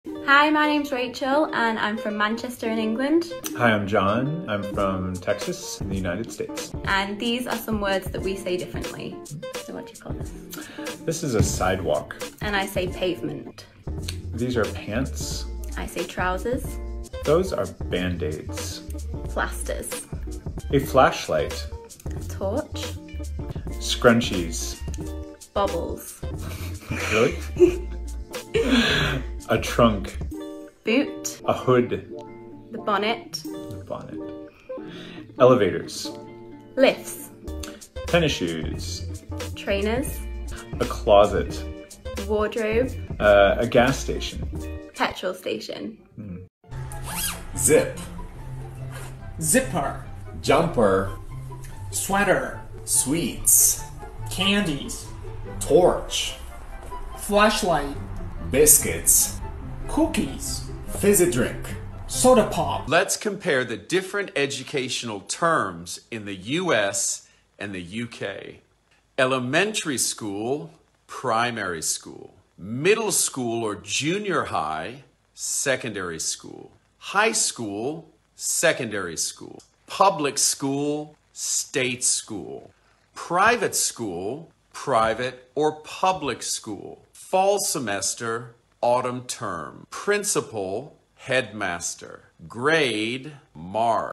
American vs. British | Different words, same meaning
Amerikan ve İngiliz İngilizcesinde aynı anlama gelen farklı kelimeler.